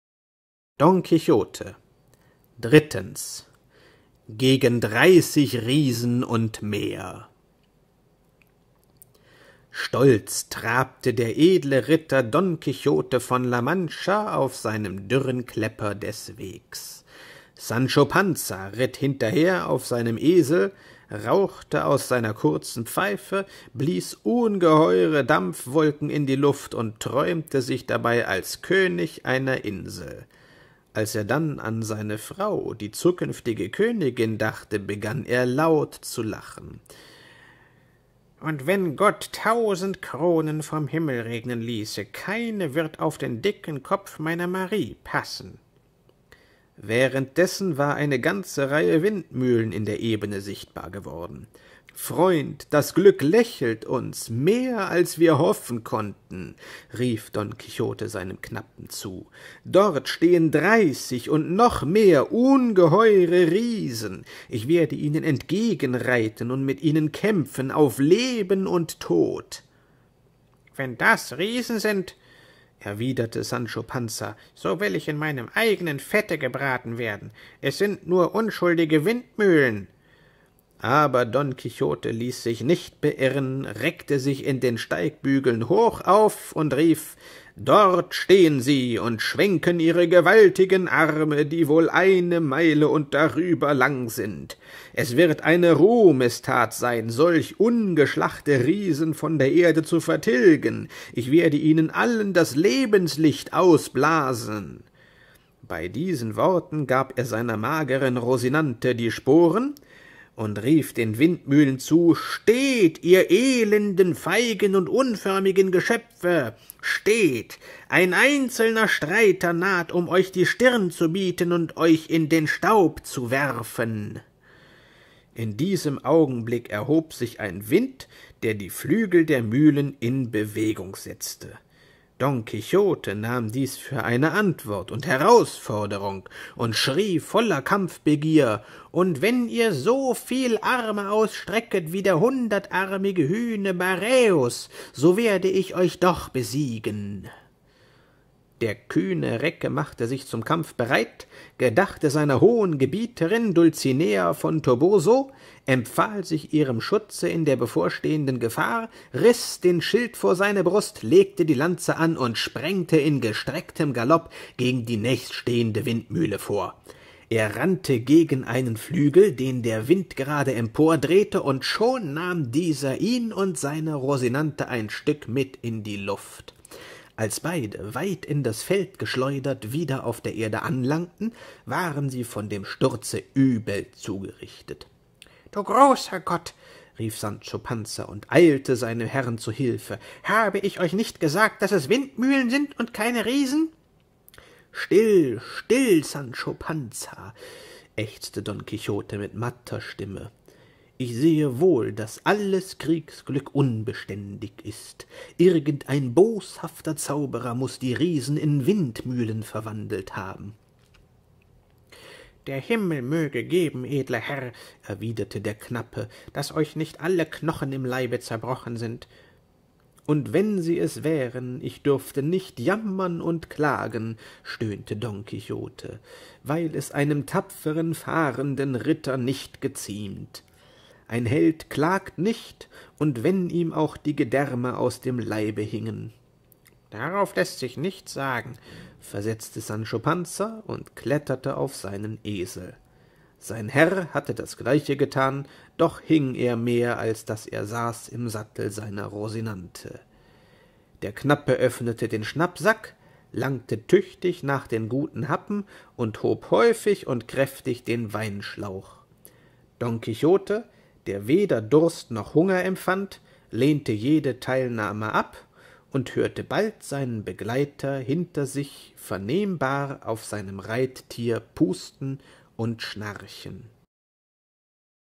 Märchen der Völker — Don Quixote: III. Gegen dreißig Riesen und mehr
Spanischer Ritterroman nach Miguel Cervantes aus dem Buch "Märchen der Völker" des Reemtsma-Bilderdienstes von 1932.
Tales of the Nations — Don Quixote: III. The Battle with the Windmills. A Spanish Tale of Chivalry from the book "Tales of the Nations" published by Reemtsma-Bilderdienst in 1932. Read in German.